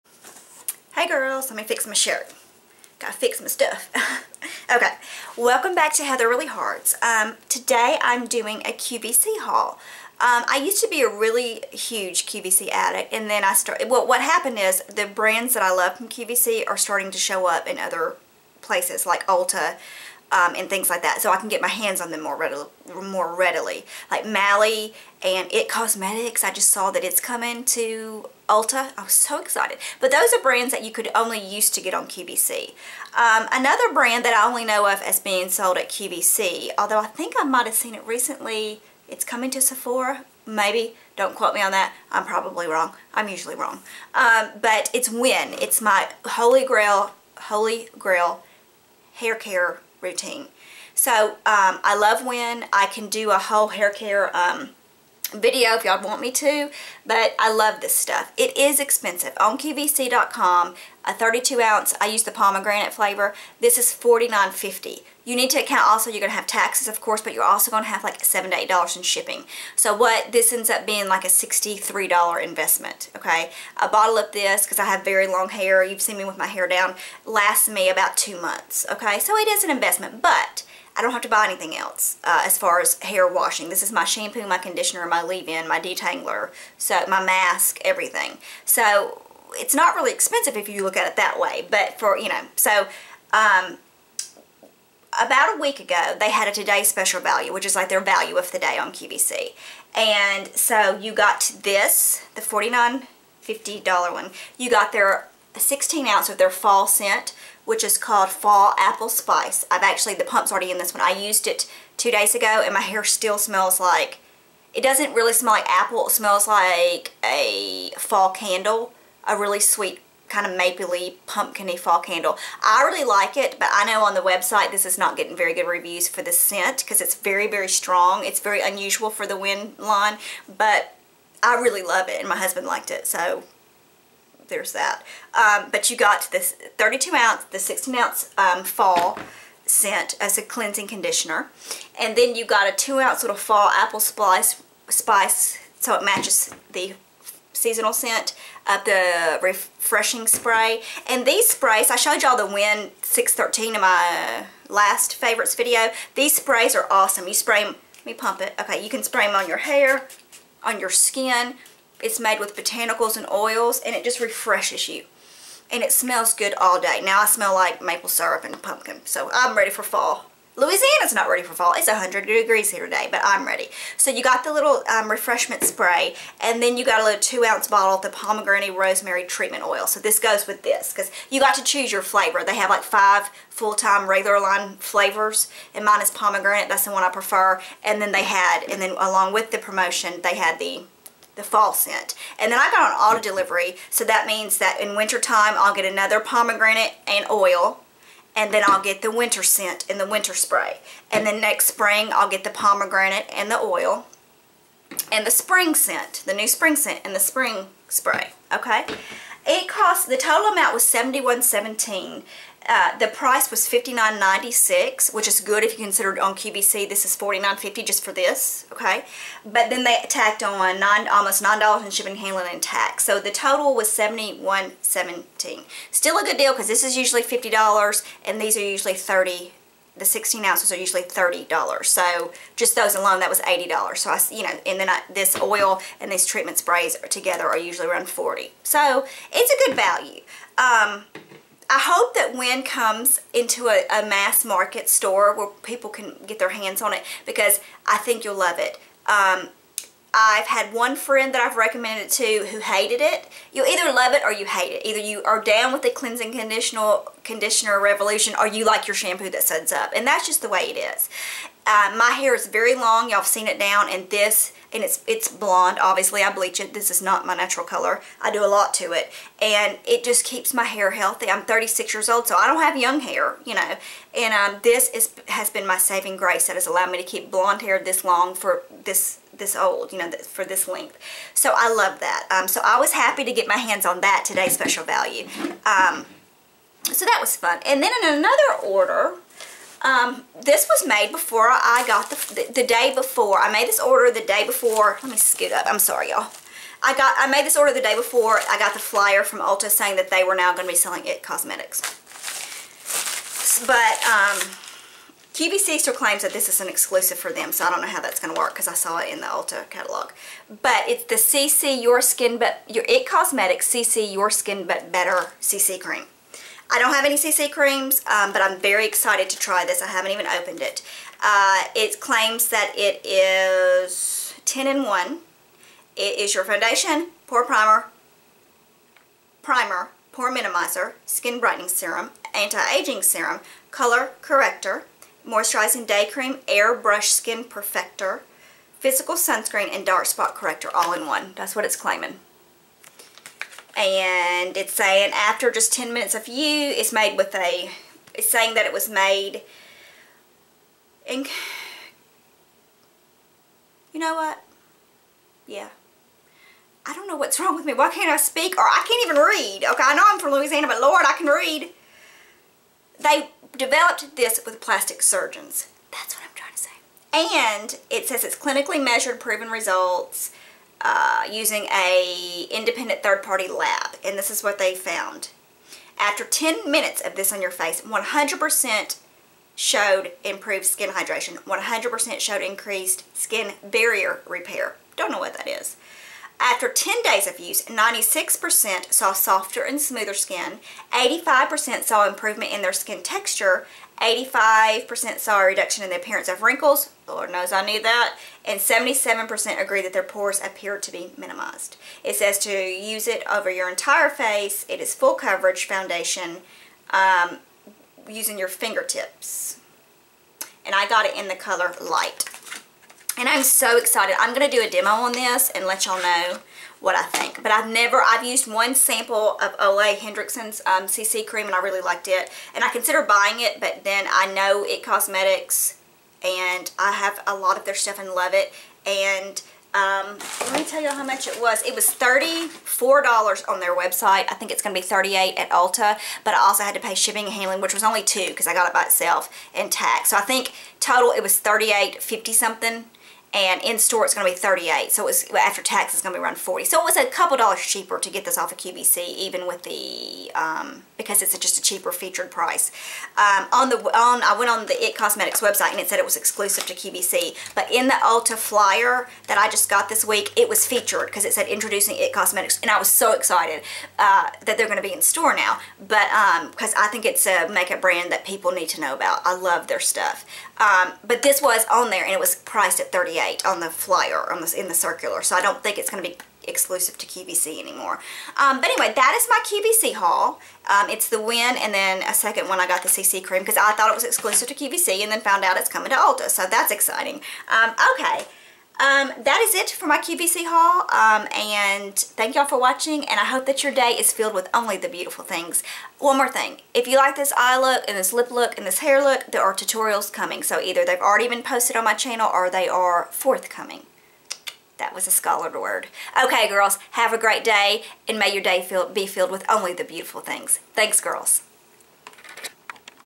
QVC Fall Haul Featuring Wen Fall Apple Spice (Limited Edition Fall Scent)
Here is a small haul from QVC Featuring my holy grail hair products from Wen.  I'm going to be doing a current hair care routine for you girls and it should be up soon!  Also, I checked after filming this video and Sephora is carrying Wen now (but only the regular 'flavors' not the limited edition seasonal 'flavors' those appear to be exclusive to QVC) ;)

Products Mentioned:
Wen Cleansing Conditioner in Pomegranate
Wen  Treatment Oil in Rosemary Pomegranate
Wen Fall Apple Spice Cleansing Conditioner (limited Edition Fall Scent)
Wen Fall Apple Spice Replenishing Spray
It Cosmetics CC+ Cream (please go watch my First Impression Demo and Review of this product I posted on Sunday)

May your Day be filled with only the Beautiful Things!